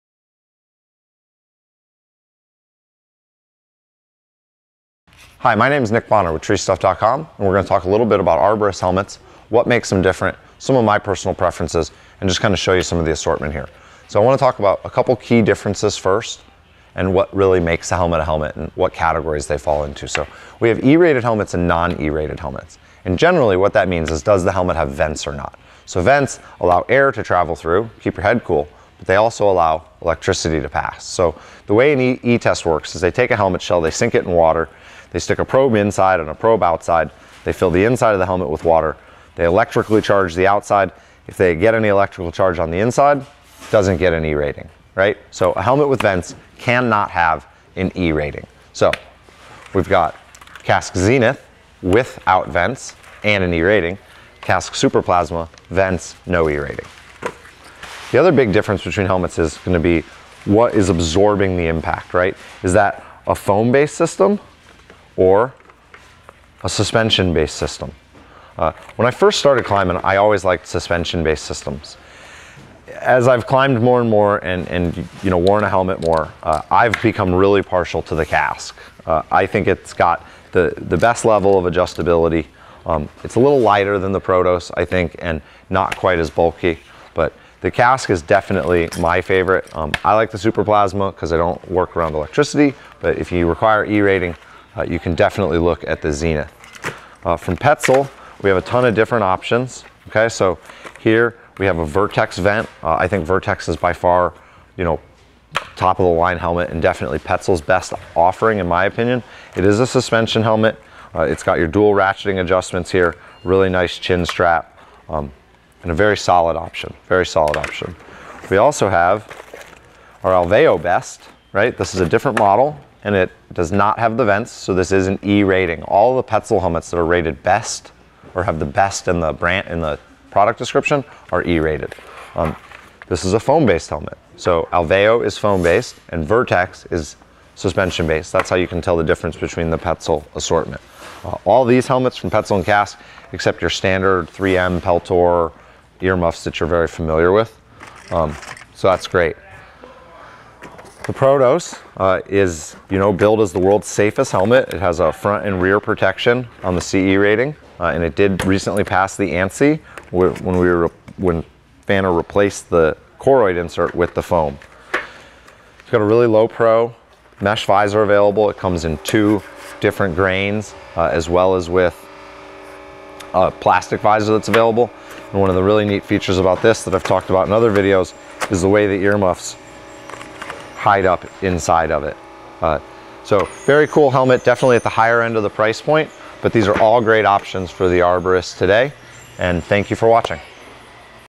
Helmets and Hard Hats Explained - What is an E Rating? - TreeStuff.com
Not every arborist helmet is made the same. Learn how to choose the best helmet for you by deciding what features you need, and which helmets offer those features in a cost-effective package.

Nick compares the electrical ratings, impact protection style, and more between the:
Kask Super Plasma
Kask Zenith
Petzl Vertex
Petzl Alveo
Pfanner Protos